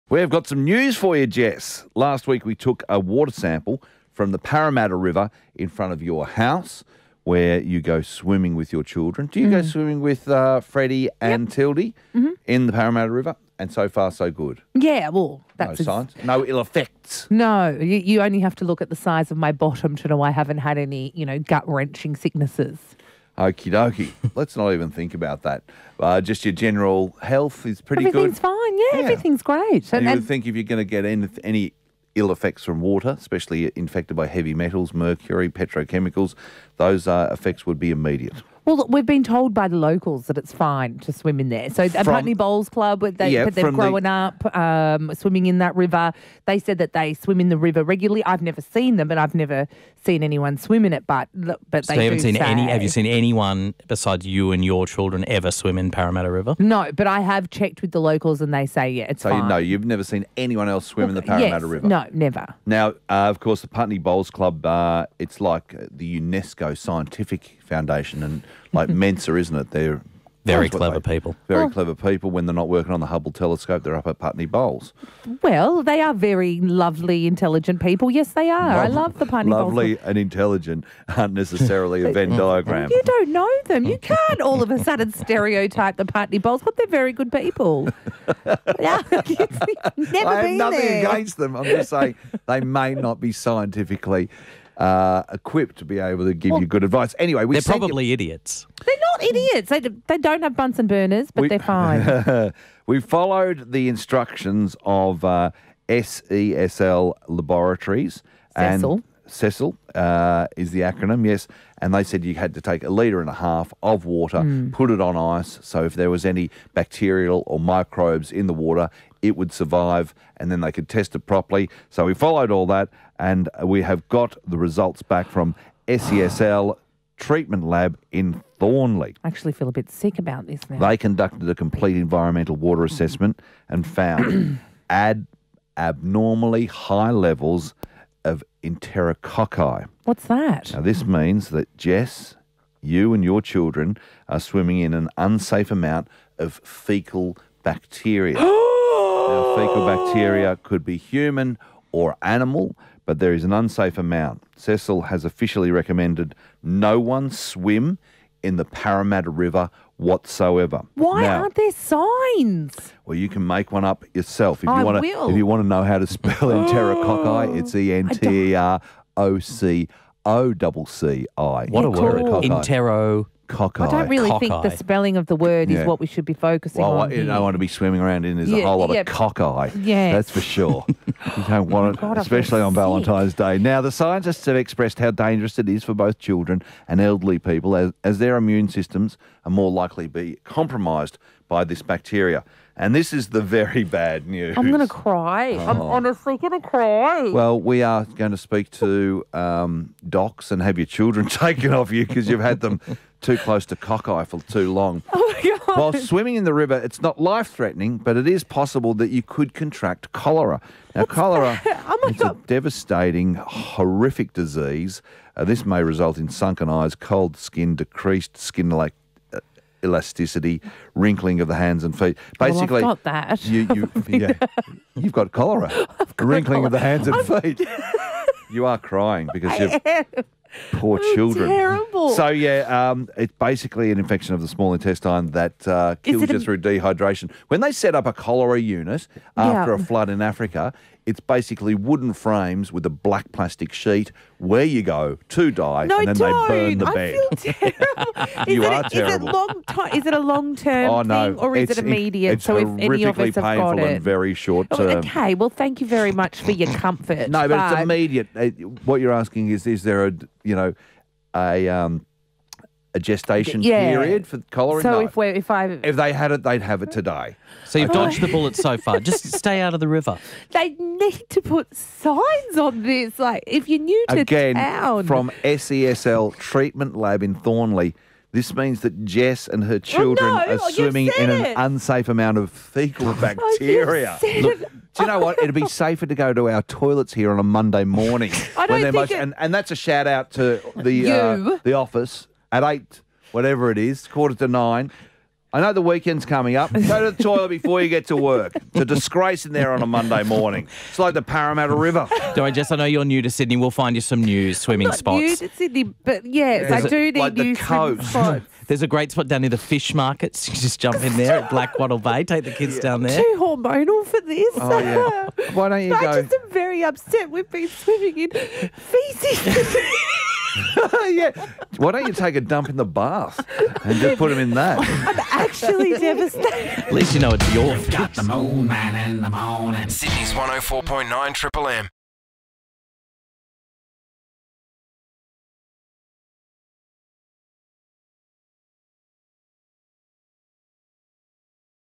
GROSS! Jess Has Parramatta River Tested And It's Scary! | Triple M
Jess was brought to tears after finding out her kids having been swimming in faeces!

Triple M brings you killer classic and the best modern rock.